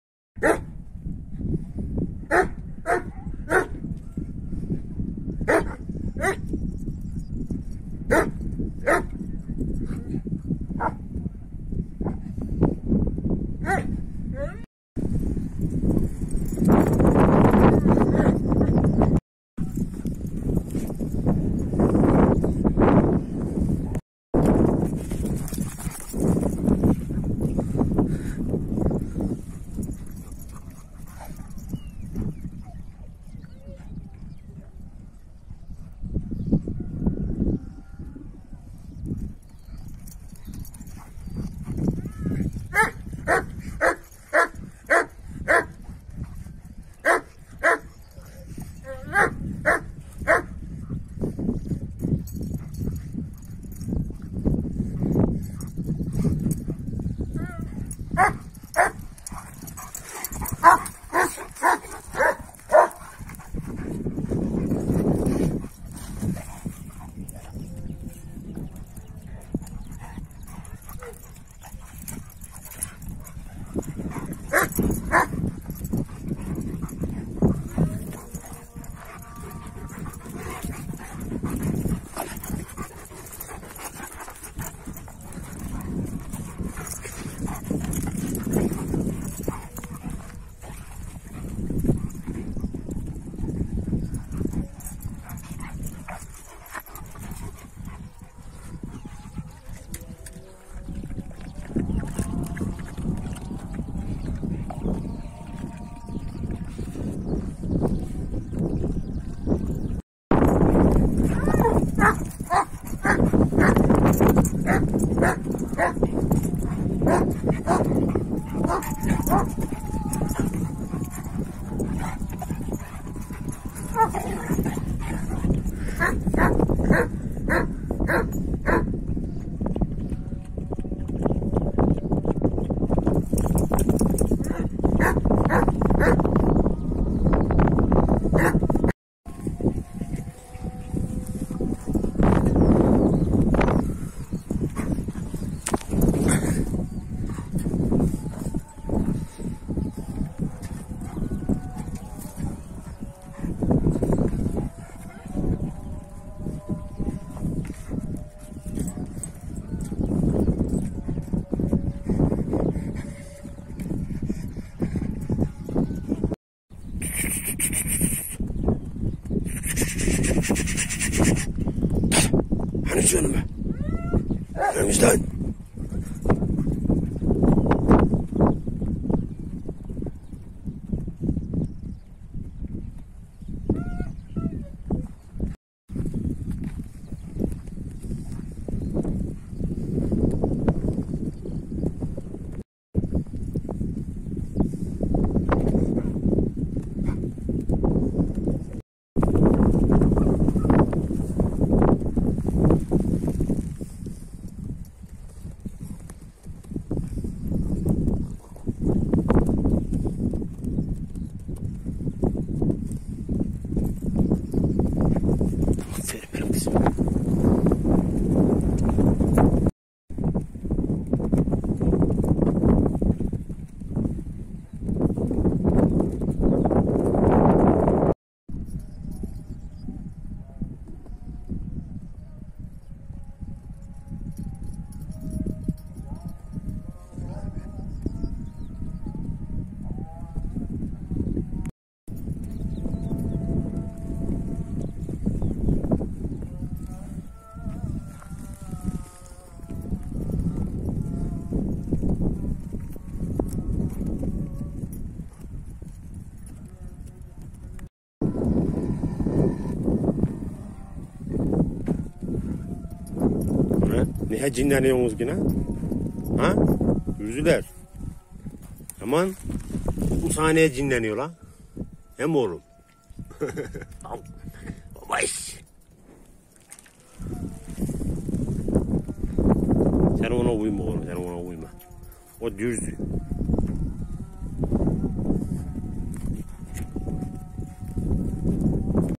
köpekler kemiği nasıl yiyor
YAMAN GÜCÜ YETMEYİNCE SİNİRLENDİ